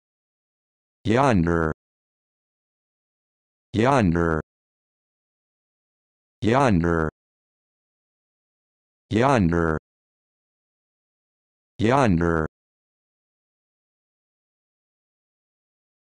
How to Pronounce Yonder
This video shows you how to pronounce Yonder